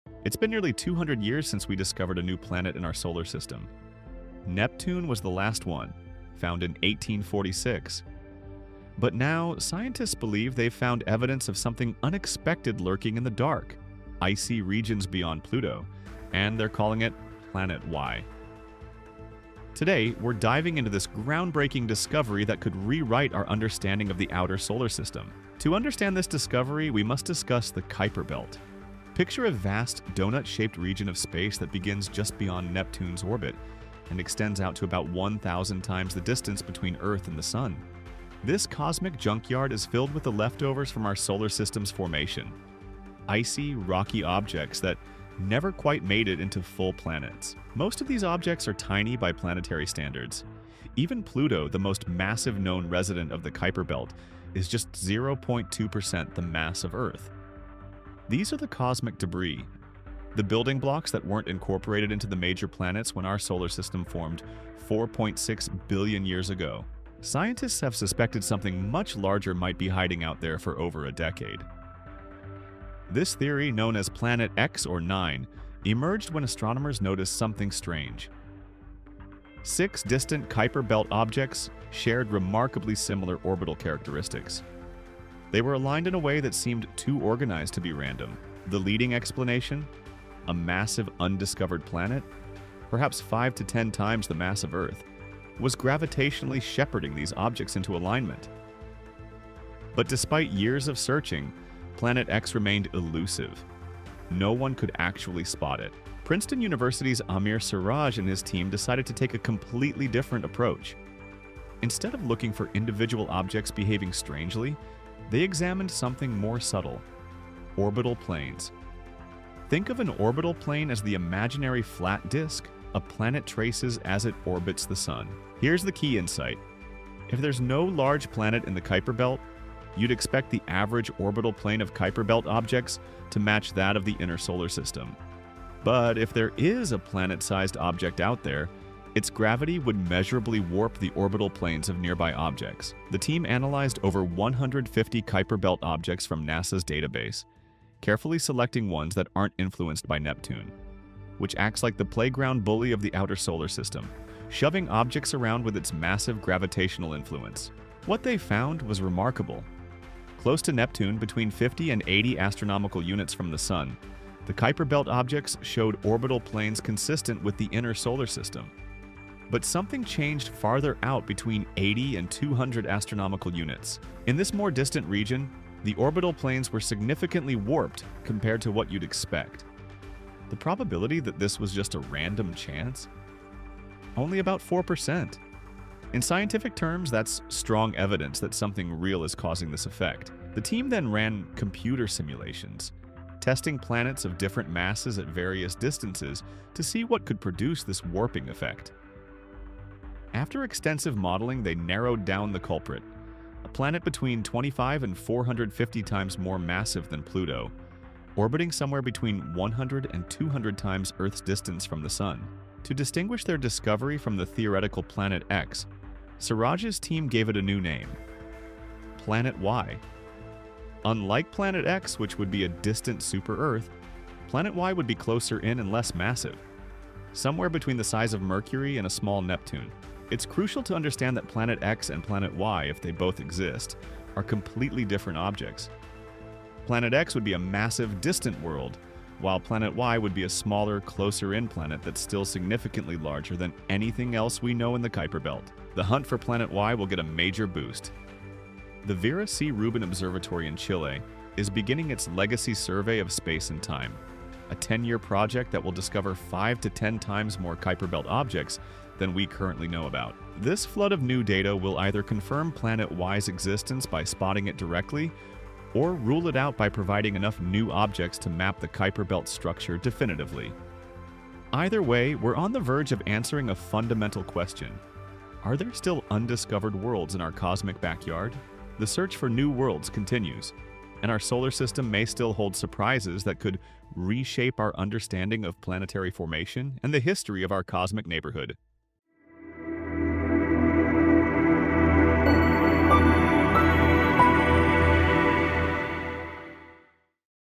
Scientists Discover Signs of Hidden Planet Y Beyond Neptune | Shocking Space Mystery Revealed
Scientists may have found evidence of a hidden Planet Y beyond Neptune. Learn what experts discovered, why it’s so mysterious, and what it could mean for our solar system.